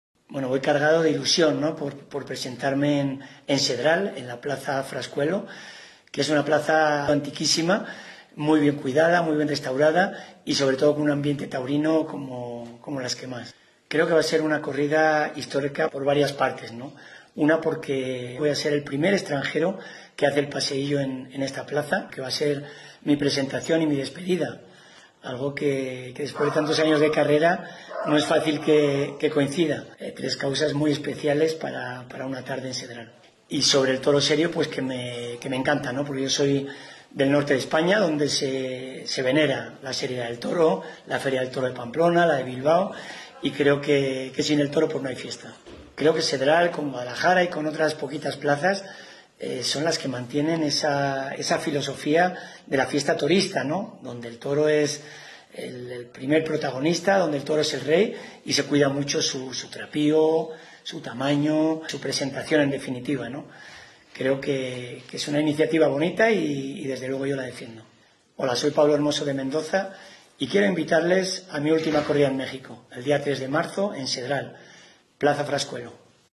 Rejoneador, Pablo Hermoso de Mendoza
‘El adiós definitivo de los ruedos mexicanos será en Cedral SLP’: Pablo Hermoso de Mendoza

Para más información visite nuestras páginas web